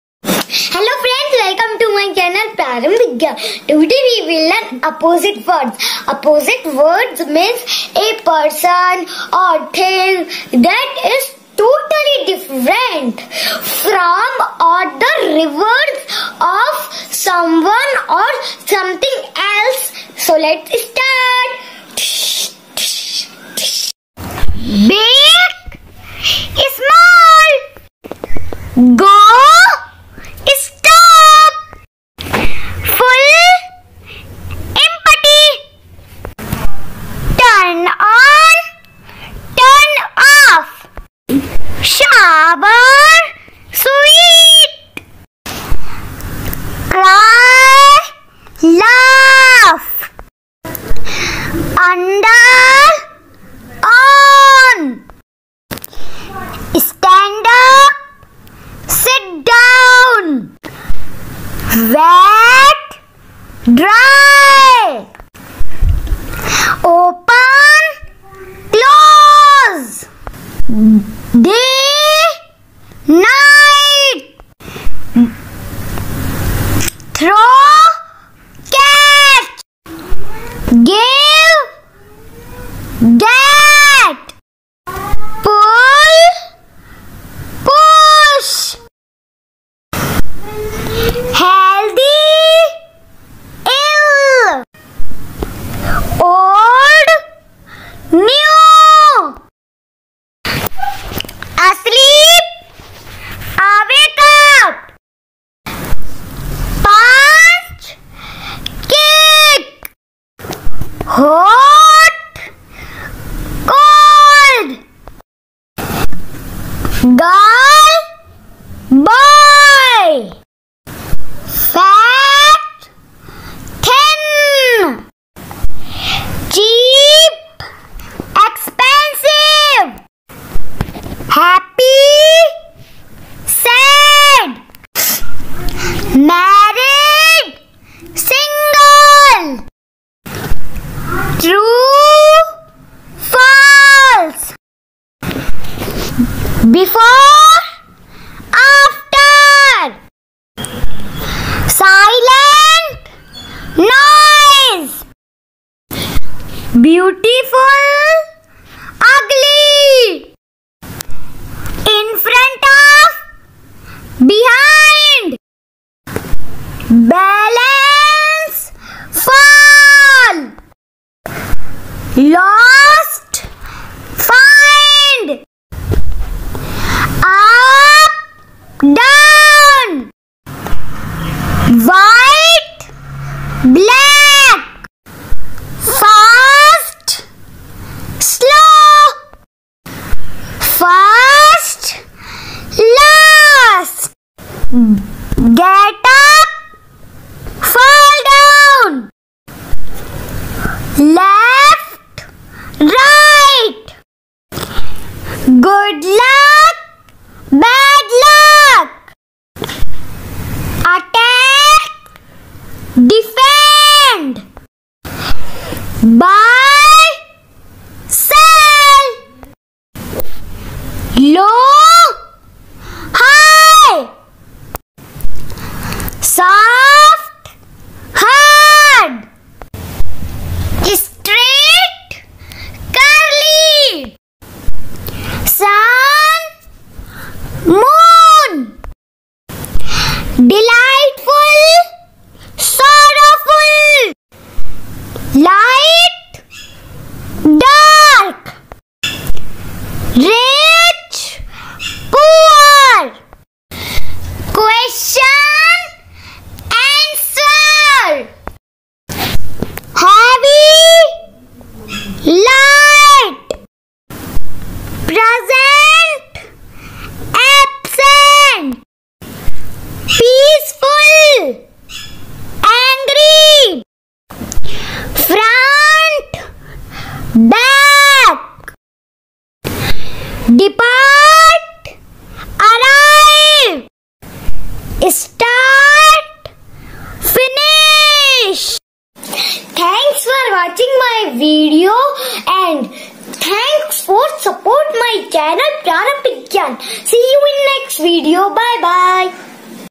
Viprit Shabd | Opposite Words | Opposite Words In English | Opposite Words For Kids
Prarambhik Gyan presenting you learning video for kids. Opositive words in English. An Opposite word is a word that Express a meaning opposed to the meaning of another word, in which case the two words are antonyms of each other. In this video, you will find the list of a oppositive words in English.